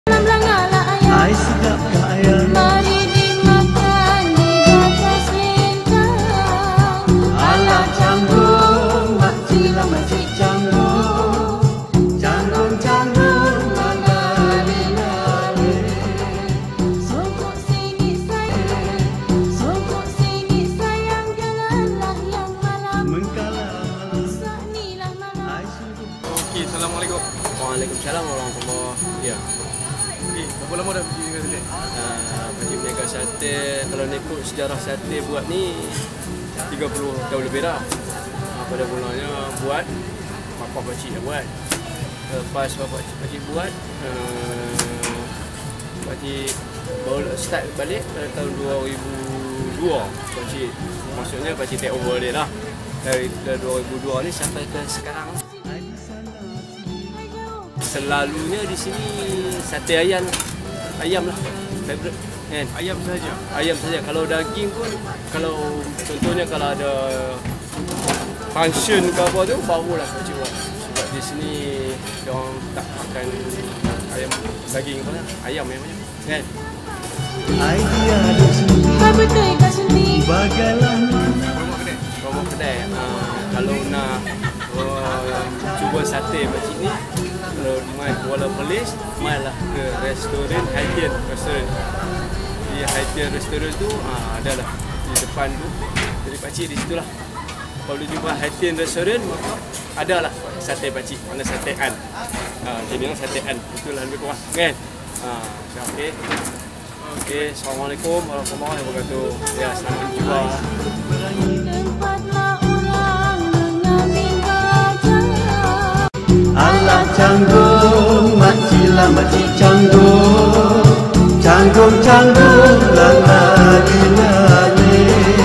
TERLALU LARIS!! SATE SEDAP DENGAN SOS KACANG LAZAT YANG HARUS DICUBA
Jika anda berkunjung ke utara Tanah Air, jangan lepaskan peluang untuk merasai keenakan Sate Ayam di sebuah restoran makanan seafood terkemuka di Kuala Perlis, Hai Thien.

Harga:  RM 0.70 / cucuk

Lokasi: Hai Thien Seafood Restaurant, Jalan Jeti Baru, 02000 Kuala Perlis, Perlis

Waktu Niaga: Setiap Hari (Rabu Cuti)
                         7.00 malam sampai habis